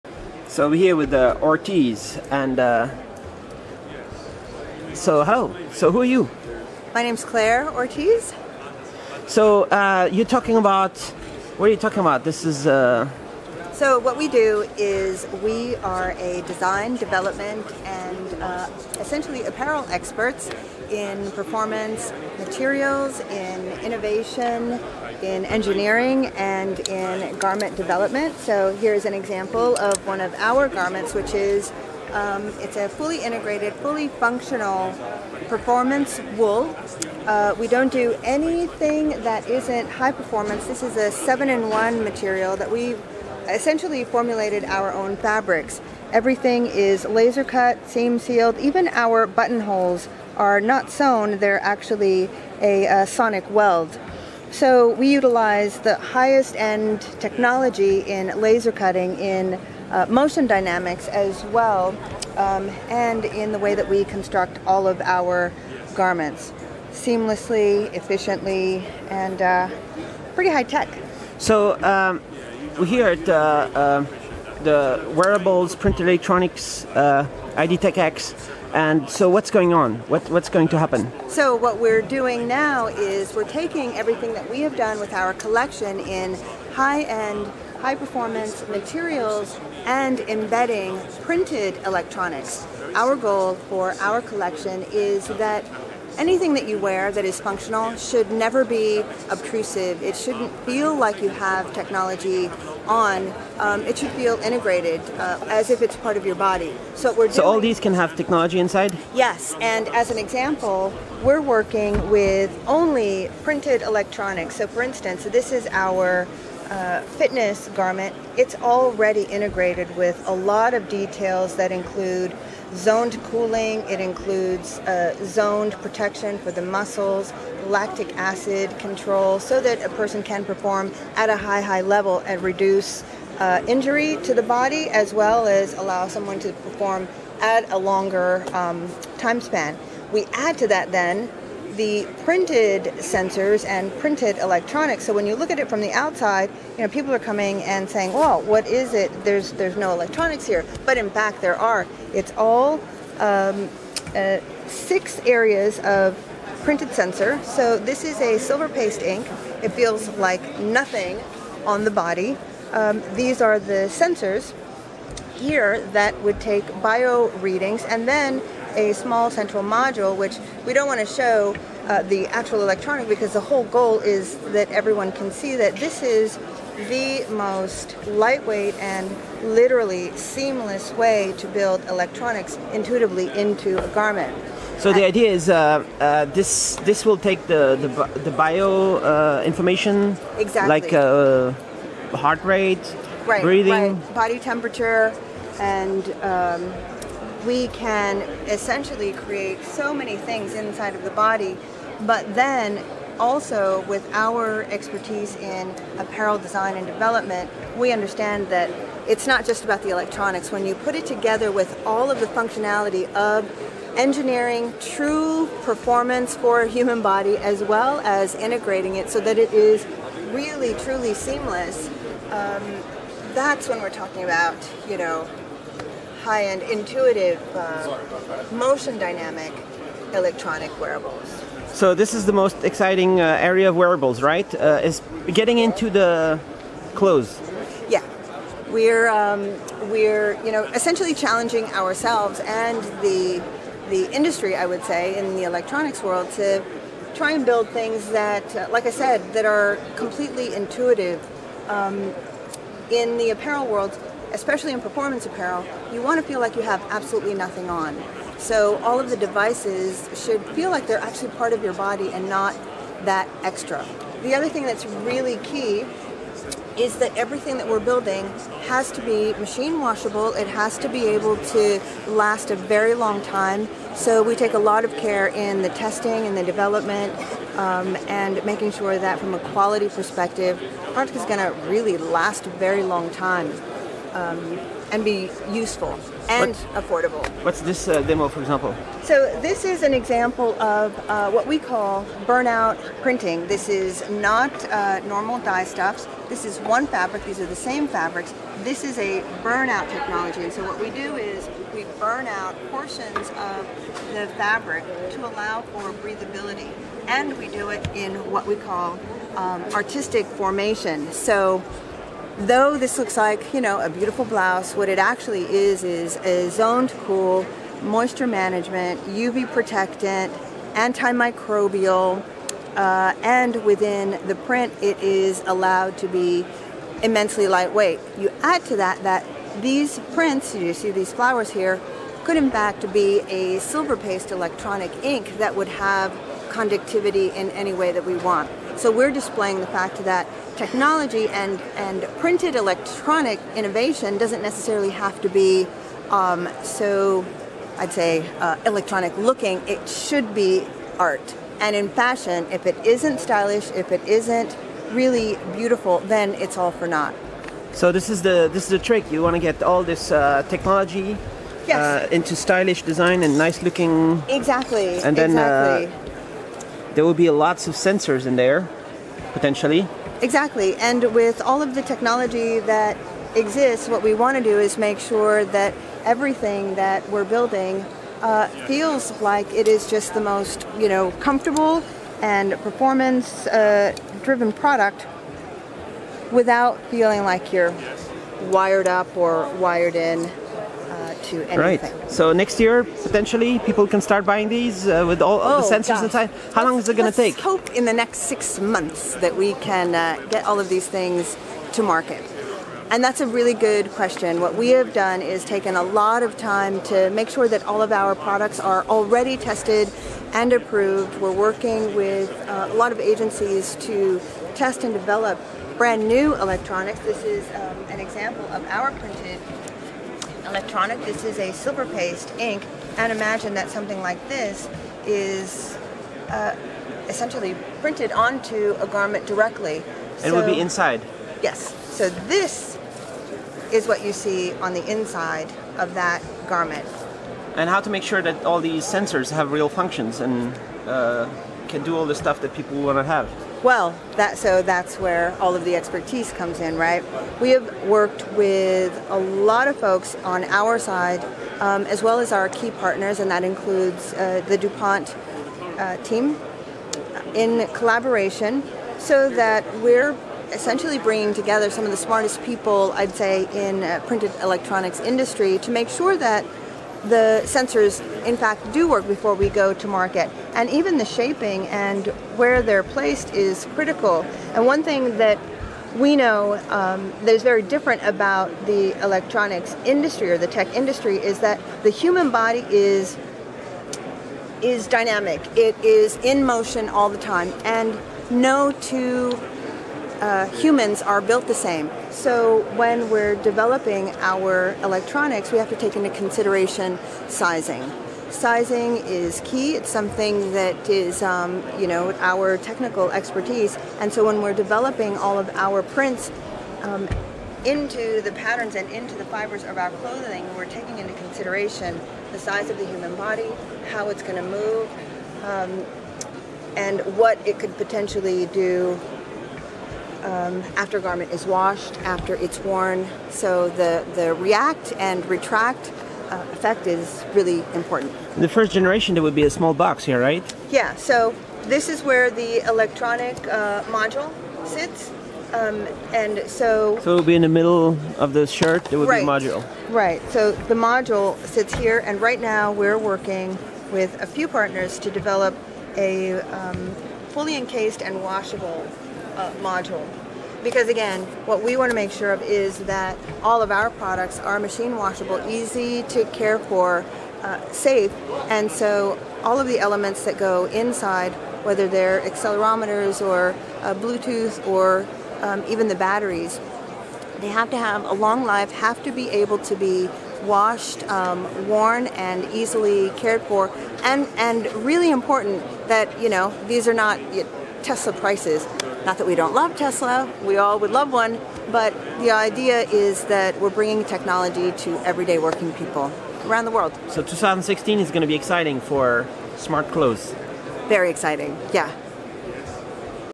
Ortiz Industry Smart Clothing
Claire Ortiz, CEO of Ortiz Industry talks at the IDTechEx Wearable USA event about their integration of advanced materials for performance apparel, including a performance sports shirt with zoned cooling and lactic acid control with incorporated printed electronics silver paste sensors working with DuPont and others to integrate smart functionalities into clothing.